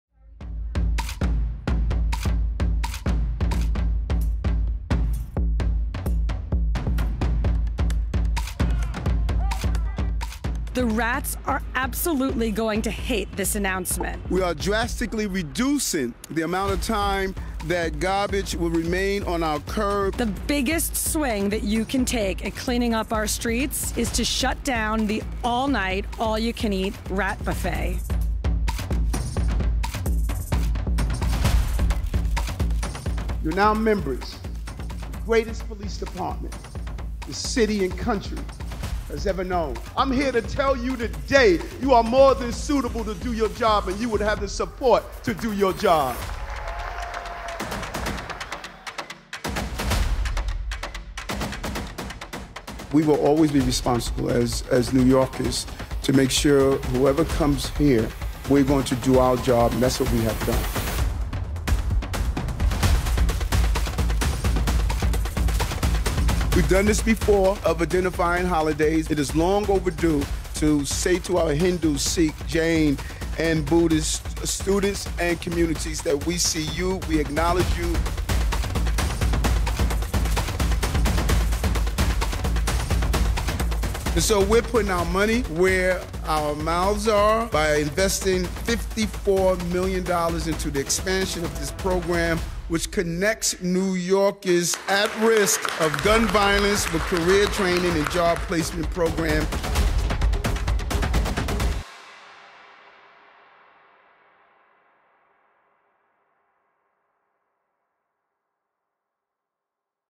Week In Review October 15 - 21
New York City celebrates Diwali, welcomes a new class of NYPD officers, creates climate jobs and brings the fight to the rats this week!